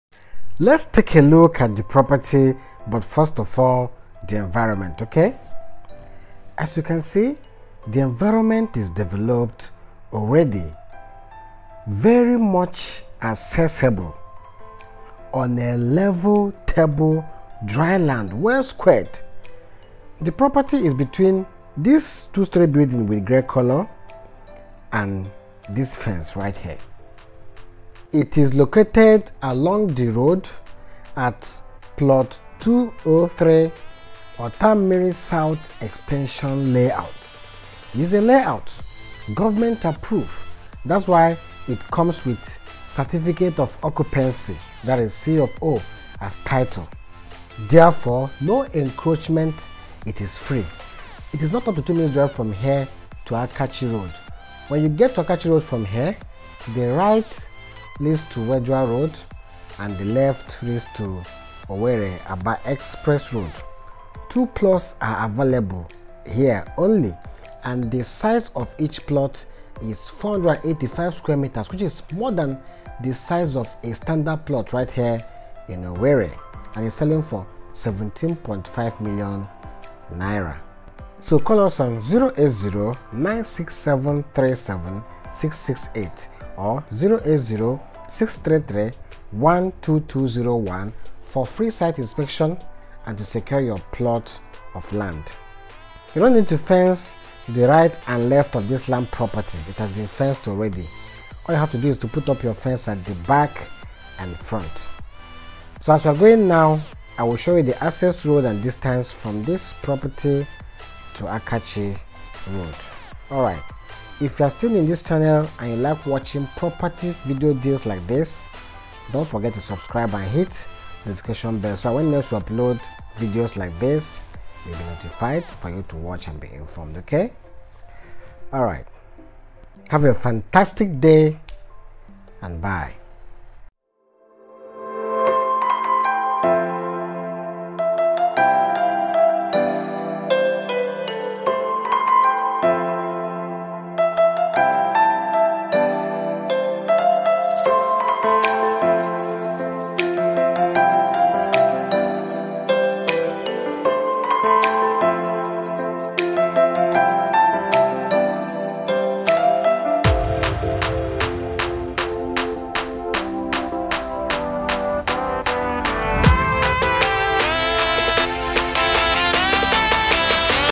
Owerri: ₦17.5M C of O Land for sale | Lands for sale in Owerri
In Owerri Town, 2mins drive to Akachi road and connects Weathral on the other side, this C of O land in available for HOT SALE!!!

The property is in Owerri town located at plot 203 Otamiri South extension layout, Owerri Imo state. It comes with certificate of occupancy(C of O) as title.
It's not up to 2mins drive to Akachi road which connects weathral road by the right side and Owerri/Aba road by the left side.
Its in a calm environment and very much accessible.

Finally, please leave me a comment - a question, correction, suggestion, and input generally - I will love to read from you.

land for sale in obosimo express highway owerri imo state